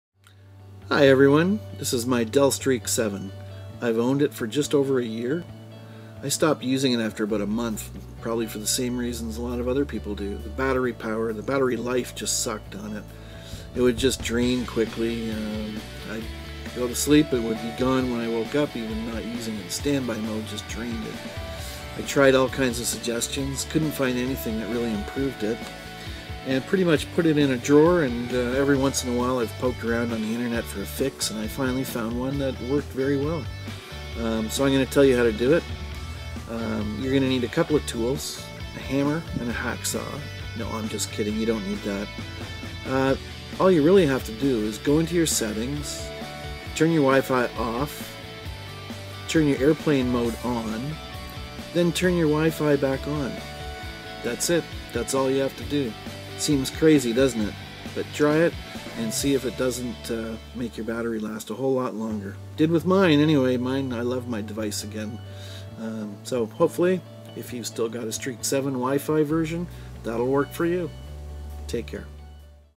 How To Extend Battery Life On Dell Streak 7 Wifi
My battery was draining overnight till I tried this. It's doubled my battery life (just going on guesstimate for that and quadrupled my standby. Just go into your settings, turn wifi off, turn airport mode on then turn wifi back on. Let me know how it works for you.